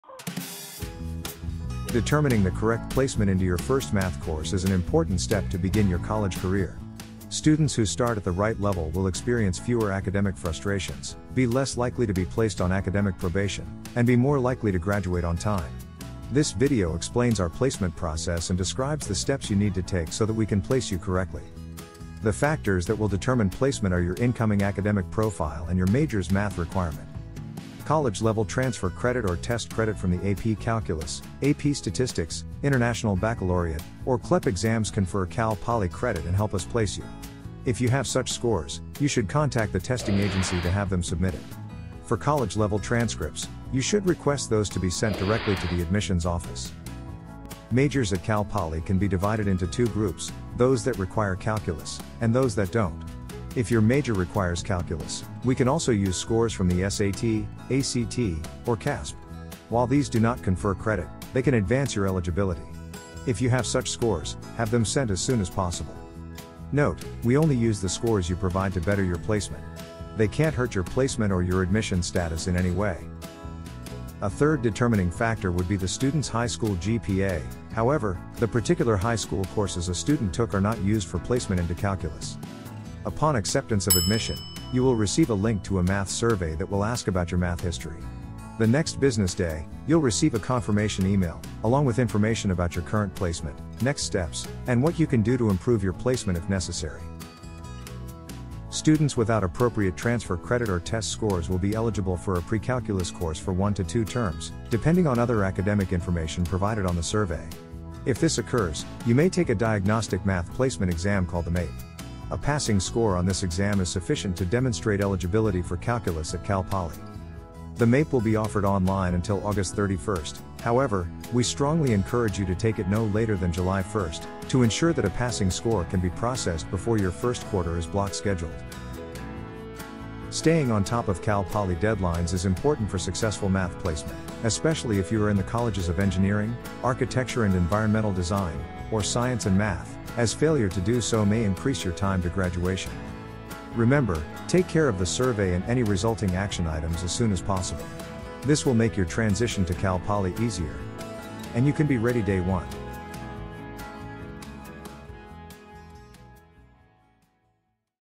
Math Placement
This video will explain how placement in mathematics is determined, as well as the options new students may have for placement based on their major.